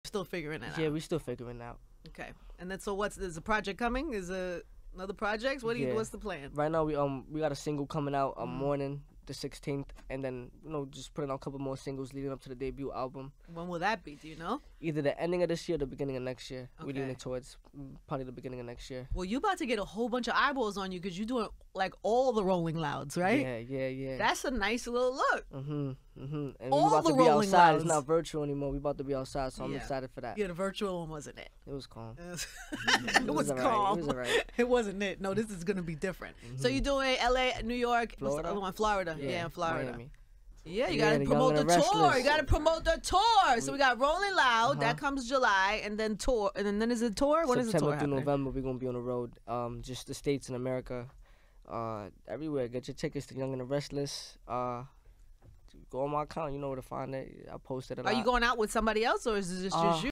J.I. the prince of N.Y. says his album will be dropping soon
might be the end of this year or next year